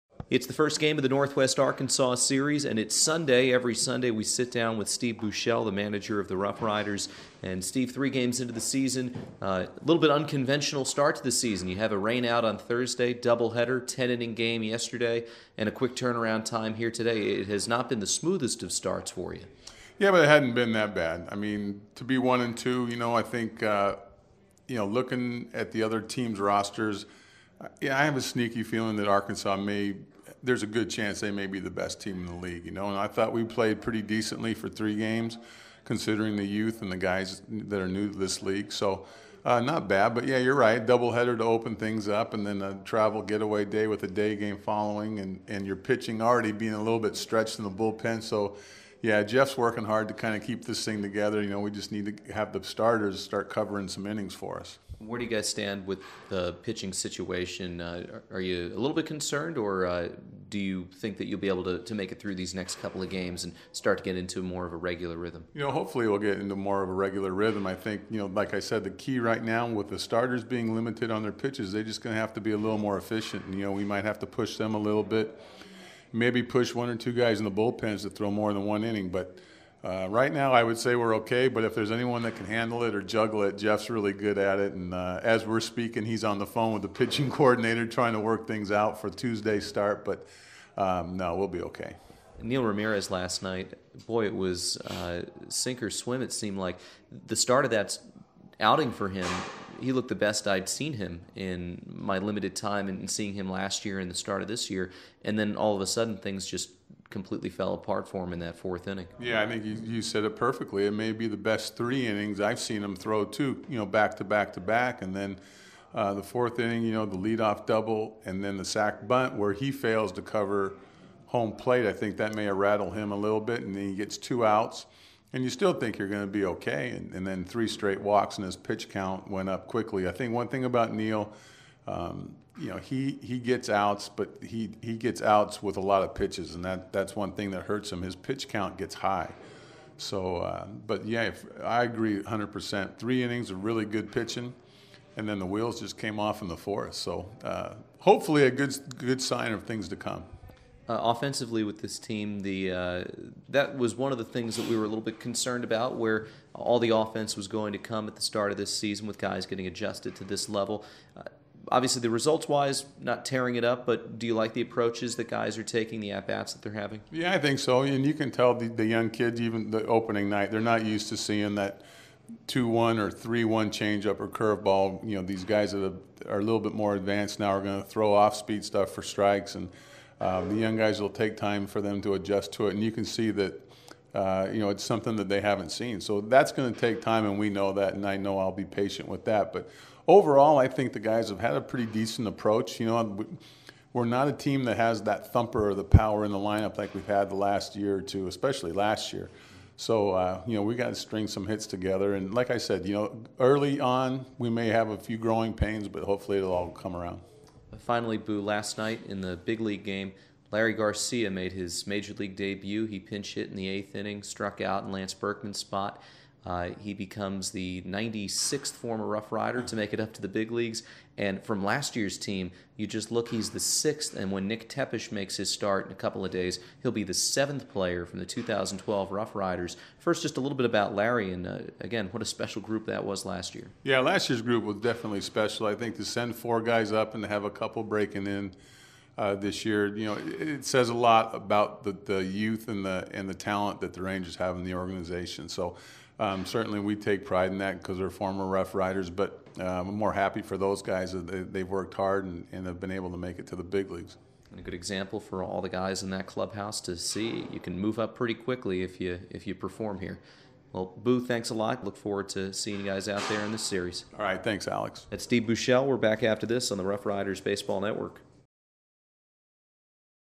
April 7, 2013 - Steve Buechele
Alex sits down again with Manager Steve Buechele. They talk about the early struggles offensively for the club, the heavily worked bullpen, and the up-and-down first start for pitcher Neil Ramirez.